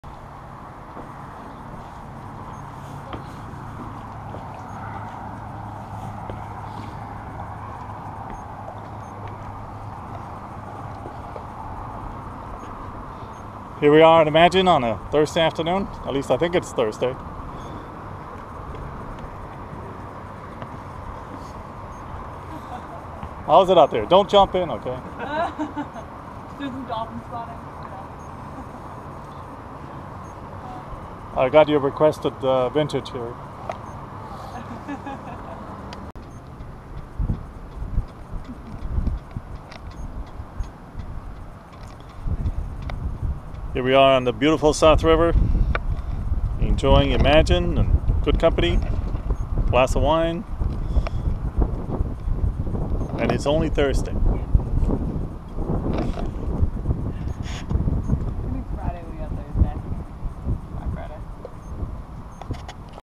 Lagoon 410 Imagine on the South River in Annapolis
A quick video from our new boat Imagine at Liberty Marina on the South River in Annapolis.